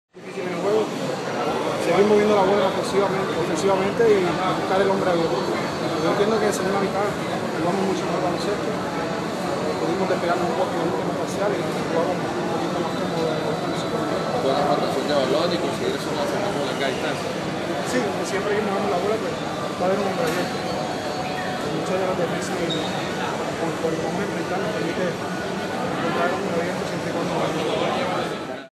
Carlos Arroyo, con experiencia en la NBA, está satisfecho con el triunfo de Puerto Rico
Carlos Arroyo, con experiencia en la NBA, está satisfecho con el triunfo de
Puerto Rico
Por Mauricio Hernández 31/08/2013